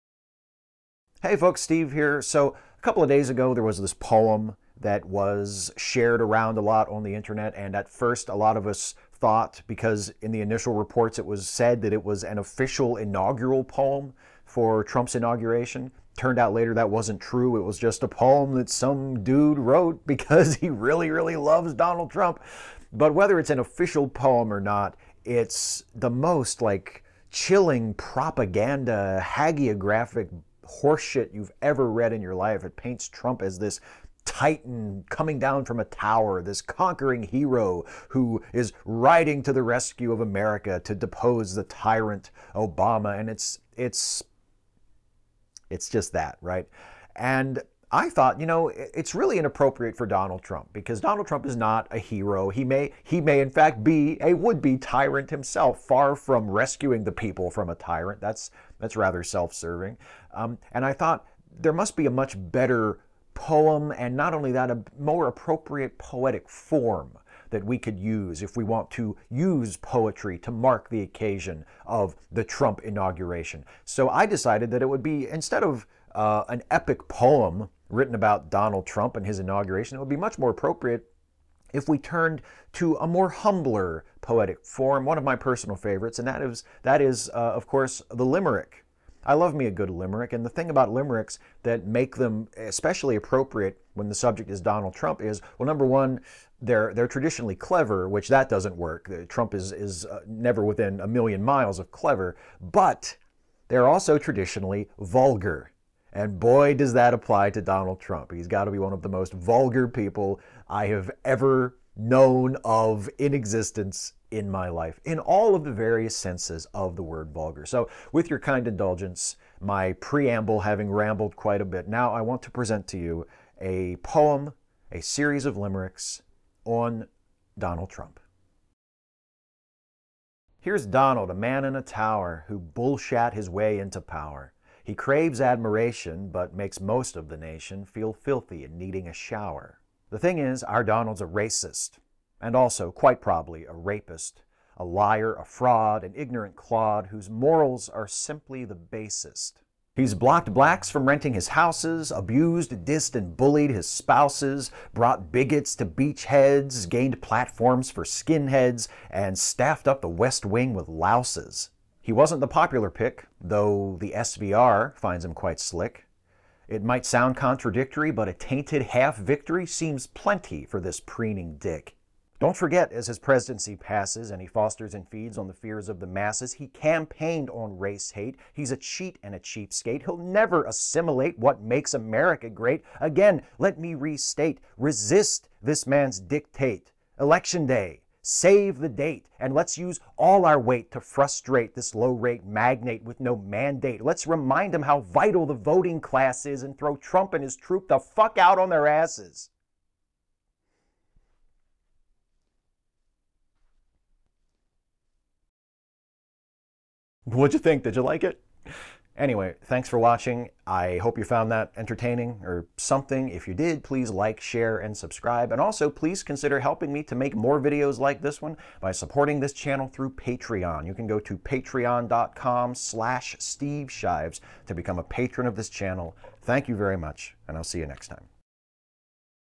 A Poem for Donald Trump
Someone wrote a poem about Donald Trump before his inauguration. So I wrote a better one.

Here’s Donald, a man in a tower,
Who bullshat his way into power.
 He craves admiration
 But makes most of the nation
Feel filthy and needing a shower.

The thing is, our Donald’s a racist,
And also quite prob’ly a rapist,
 A liar, a fraud,
 An ignorant clod,
Whose morals are simply the basest.

He’s blocked Blacks from renting his houses,
Abused, dissed and bullied his spouses,
 Led bigots to beachheads,
 Gained platforms for skinheads,
And staffed-up the West Wing with louses.

He wasn’t the popular pick,
Though the SVR finds him quite slick.
 It might sound contradict’ry
 But a tainted half-vict’ry
Seems plenty for this preening dick.

Don’t forget as his presidency passes,
And he fosters and feeds on the fears of the masses,
 He campaigned on race hate,
 He’s a cheat and a cheapskate,
 He’ll never assimilate
 What makes America great
 Again – let me restate:
 Resist this man’s dictate.
 Election Day: save the date
 And let’s use all our weight
 To frustrate this low-rate
 Magnate with no mandate
Let’s remind ‘em how vital the voting class is
And kick Trump and his troop the fuck out on their asses.

Thanks!